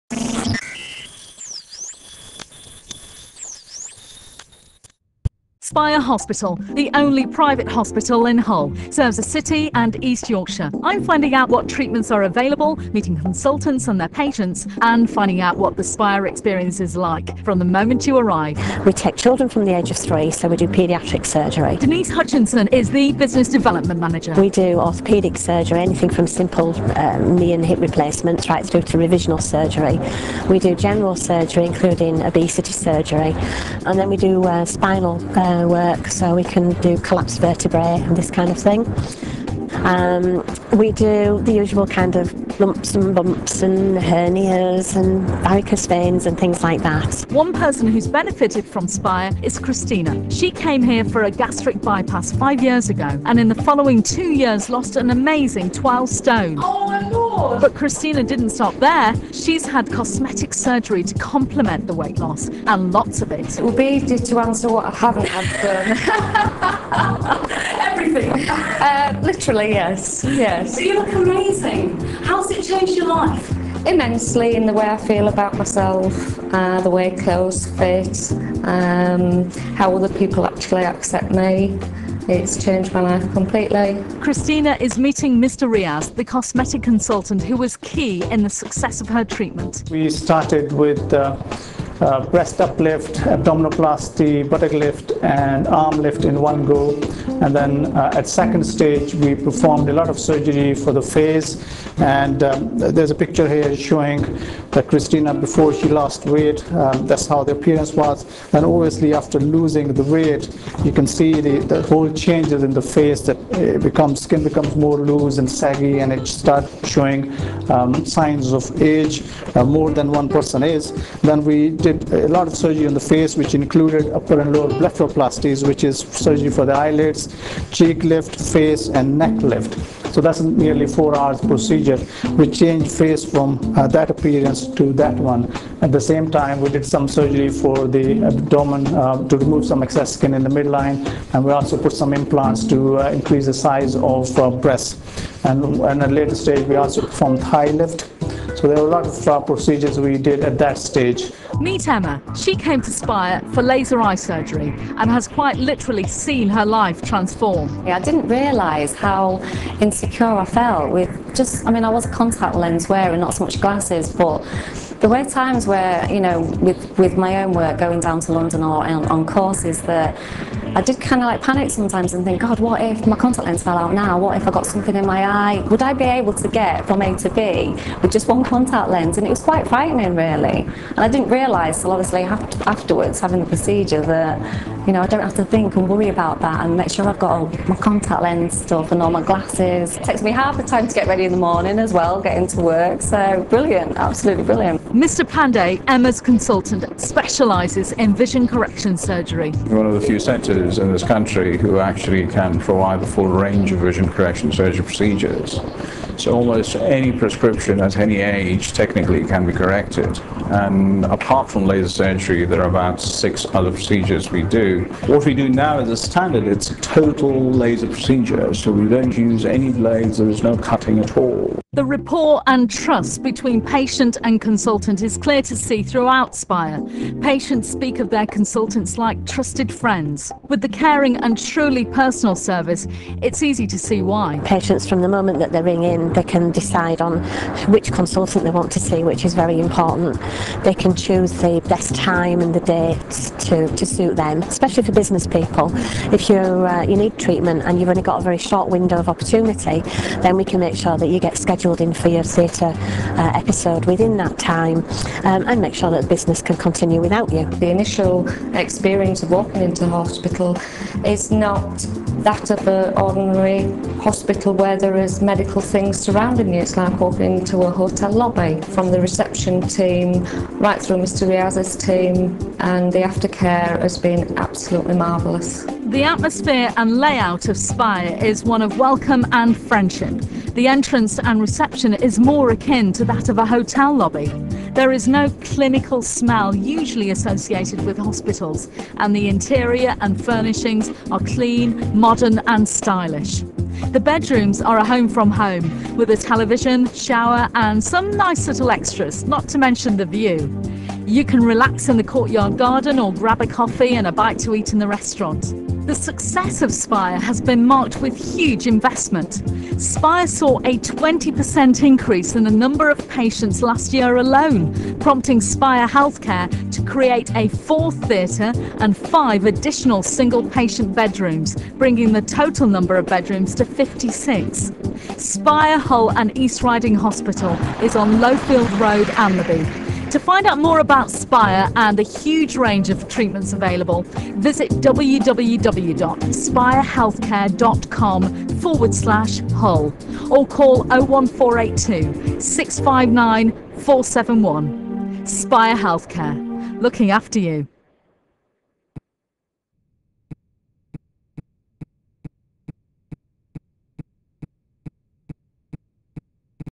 Spire Hull & East Riding Hospital
Viking FM TV's Video highlighting the treatments and facilities at Spire Hull & East Riding Hospital, Hull's only Private Hospital. Presented by Samantha Rowlands, filmed and produced by Patrick Billington.